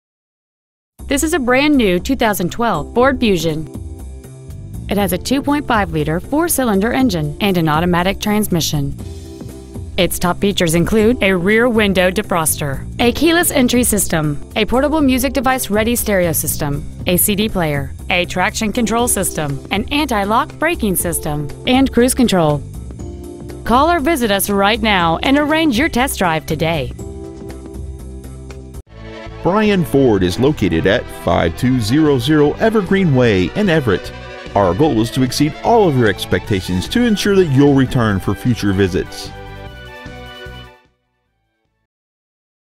2012 Ford Fusion Everett WA 98203
Year : 2012
 Make : Ford
 Model : Fusion
 Engine : 2.5L I4
 Trans . : Automatic
 Exterior : Ingot Silver
 Miles : 25
 Interior : Light Stone
 Brien Ford
 5200 Evergreen Way
 Everett , WA 98203
 2012 Ford Fusion Everett WA